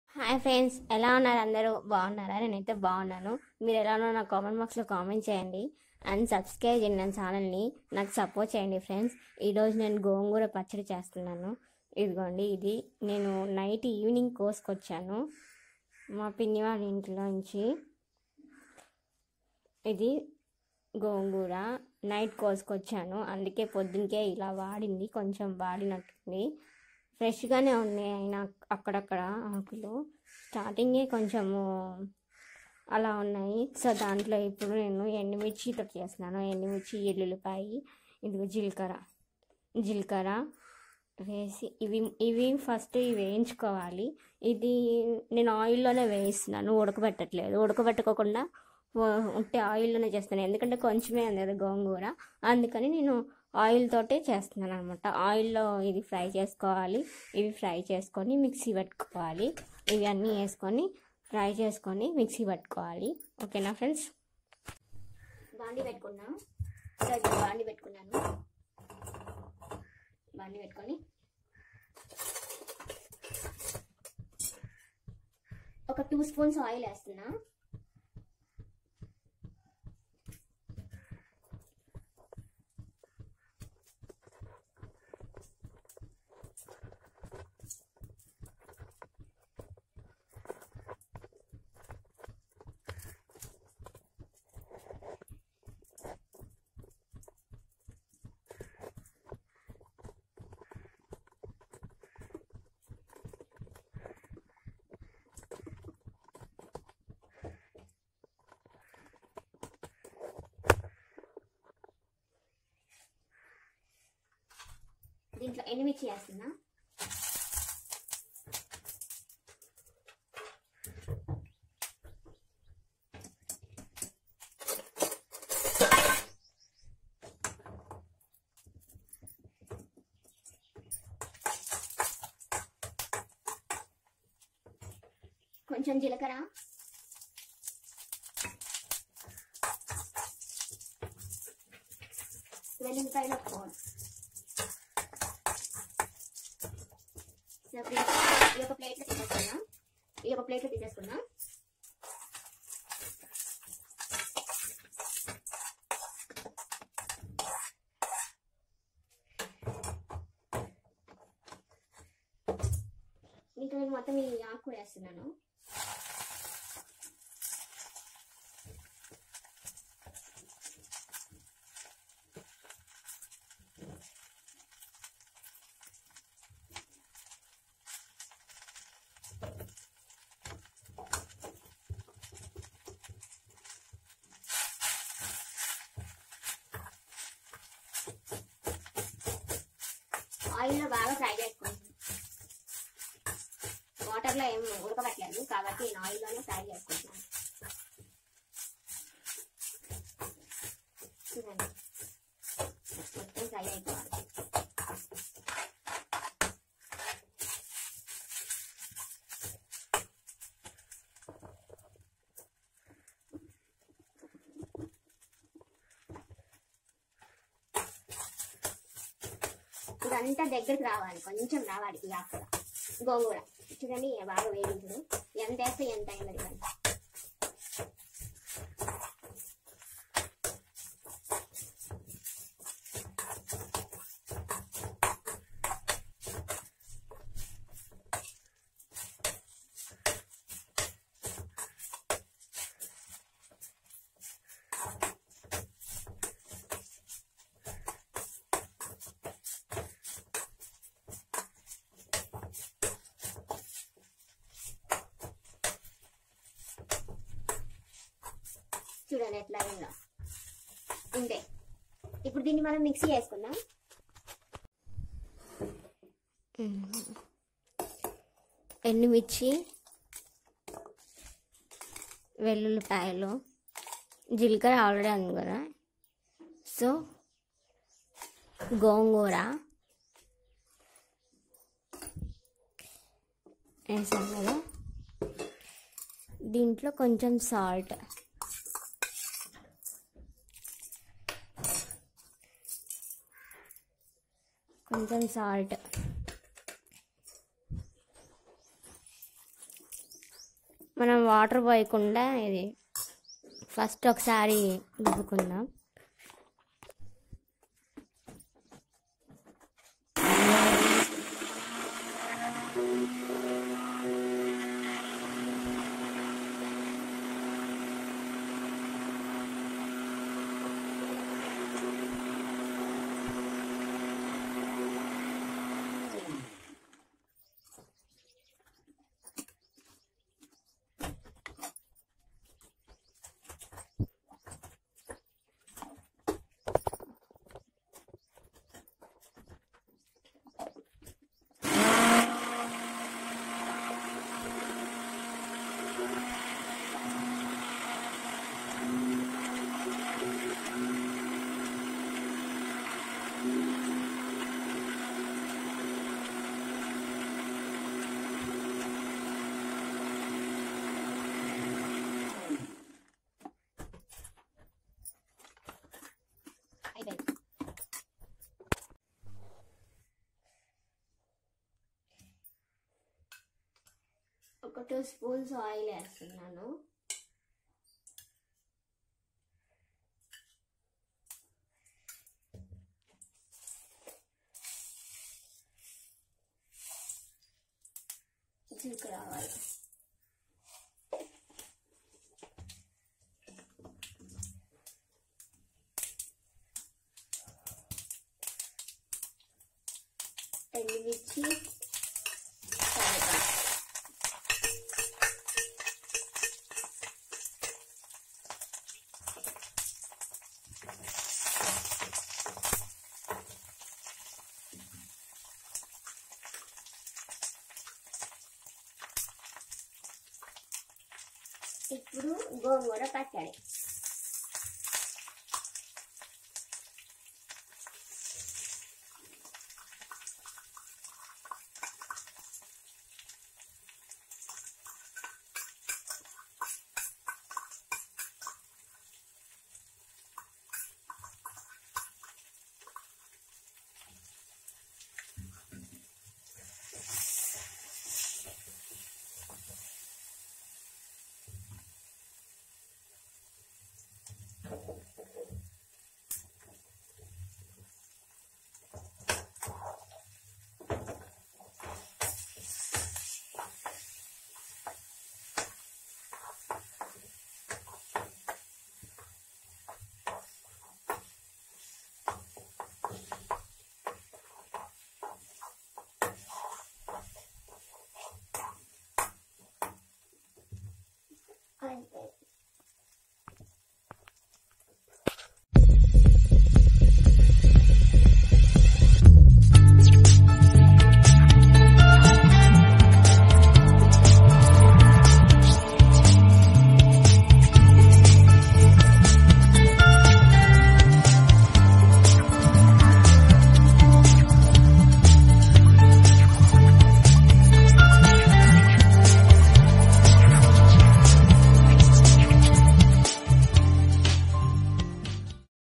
How to make gongura pickle 😋 traditional pickle Andra style 😋గోంగూర పచ్చడి తయారీ విధానం 😋
Andra style 😋 గోంగూర పచ్చడి తయారీ విధానం 😋